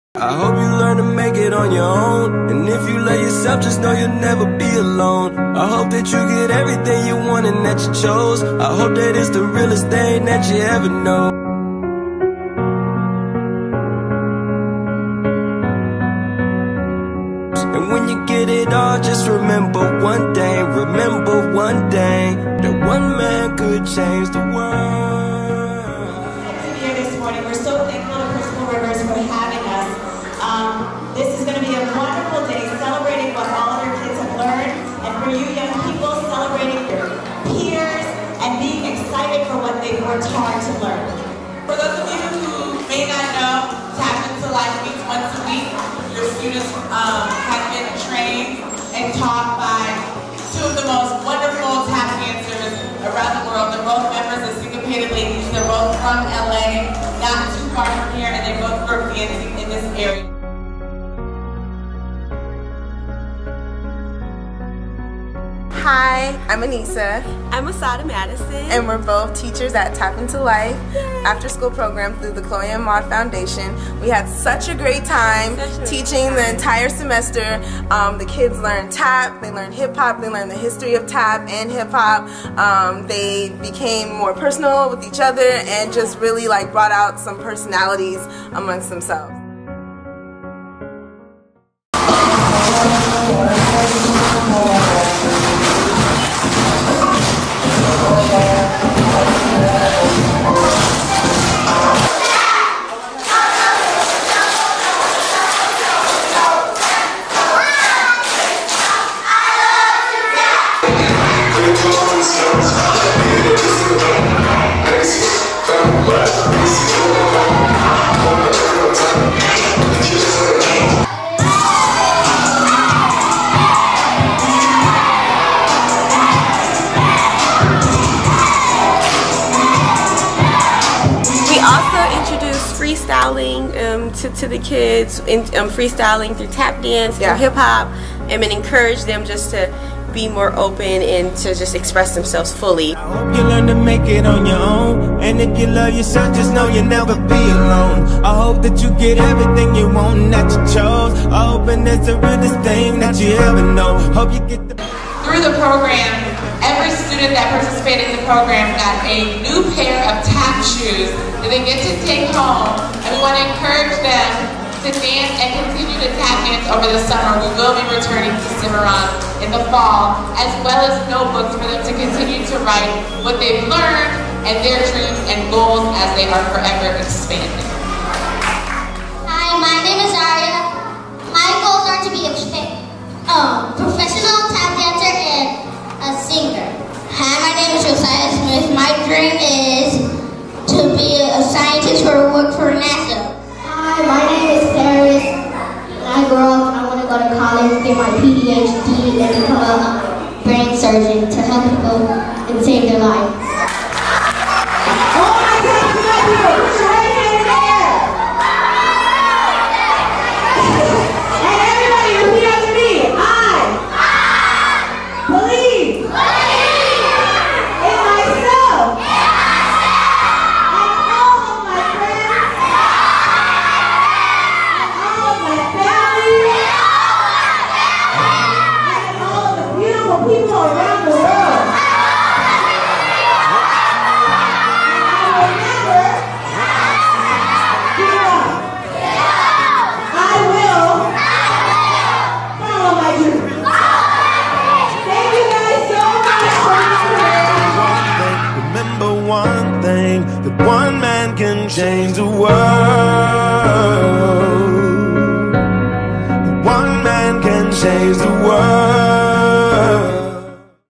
Chloe & Maud Foundation - Tap Into Life Culminating Show
TAP INTO LIFE - A free for kids after school program to learn Tap Dance, Hip Hop, and Entrpreneurial skills